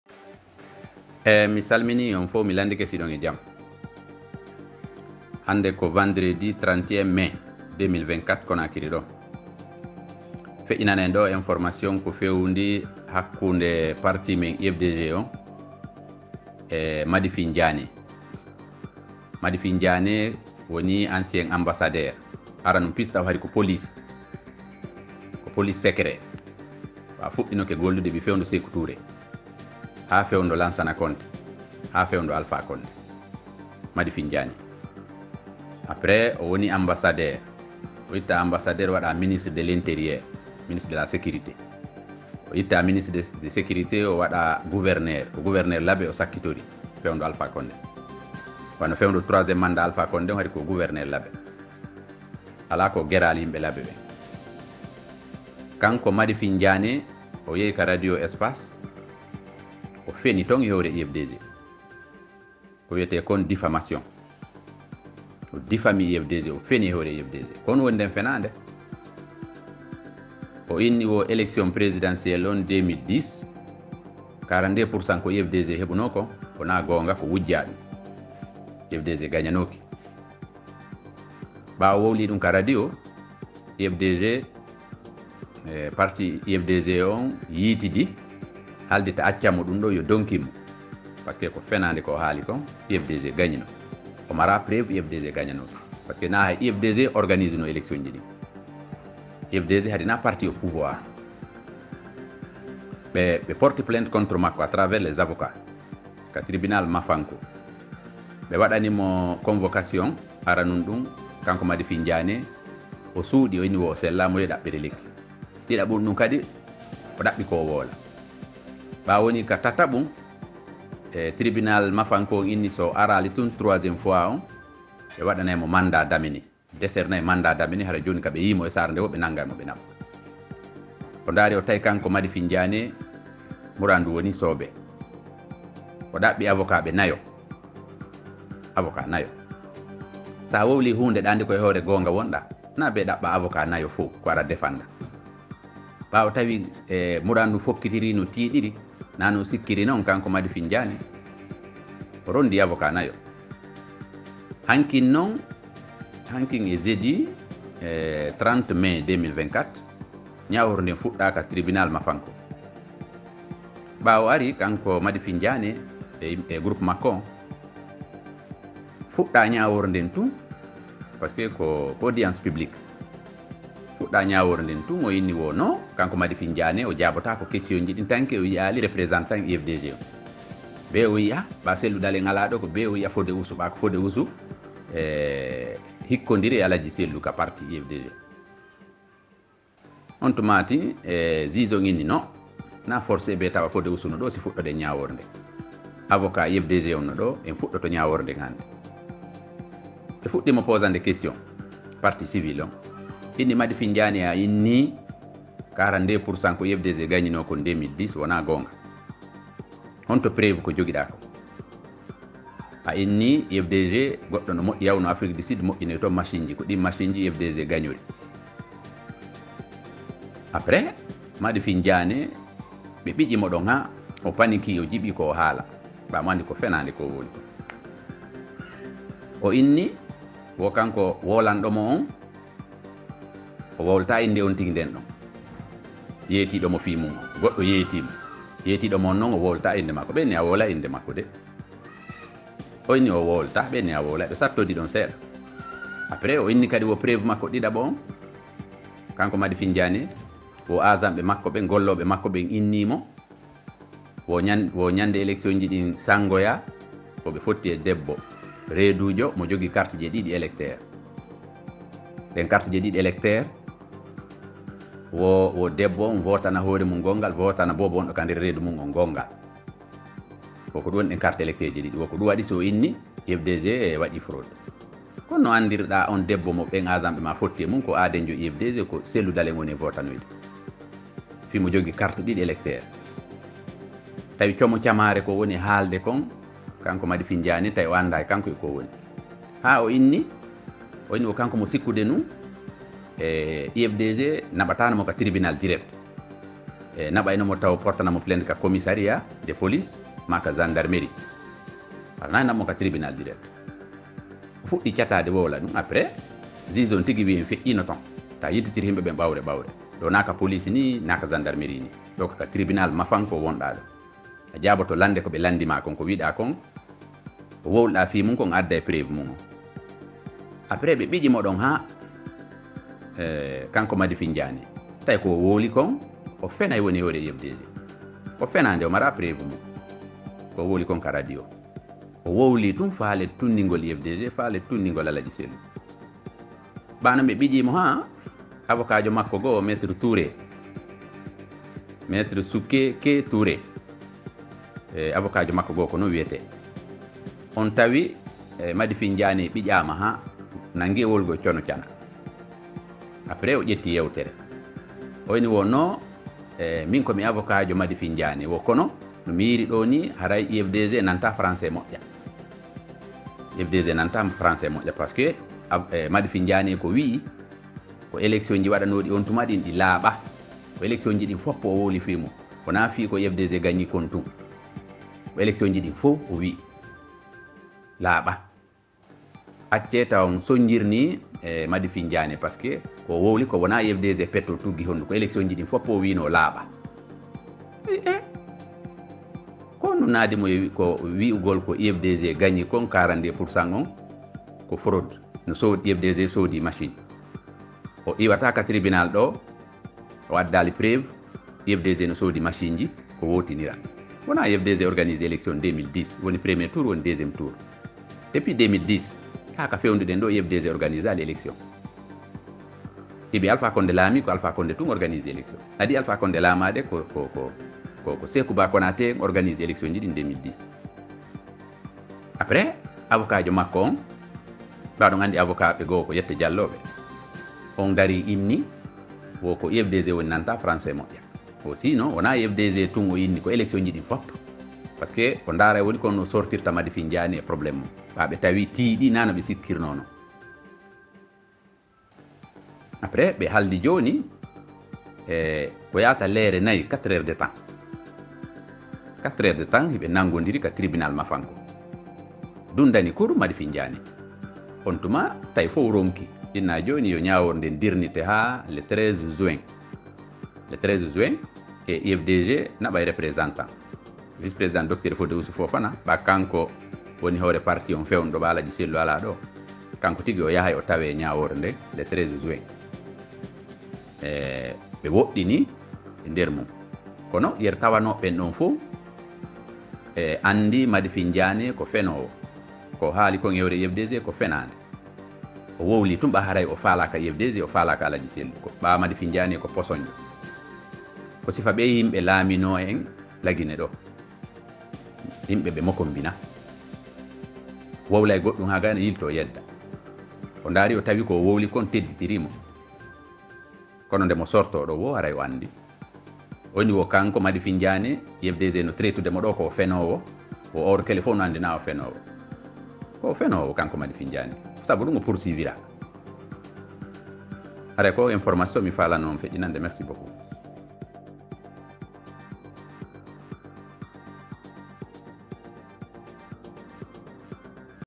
l'affaire opposant le parti Union des forces démocratiques de Guinée (UFDG) à l'ex-ms de la Sécurité
Le procès dans l'affaire opposant le parti Union des forces démocratiques de Guinée (UFDG) à l'ex-ministre de la Sécurité, Madifing Diané, s'est tenu ce jeudi 30 mai 2024, au Tribunal de première instance de Mafanco. Après des débats houleux entre les parties au procès, le Tribunal a renvoyé l'audience au 13 juin prochain pour la suite.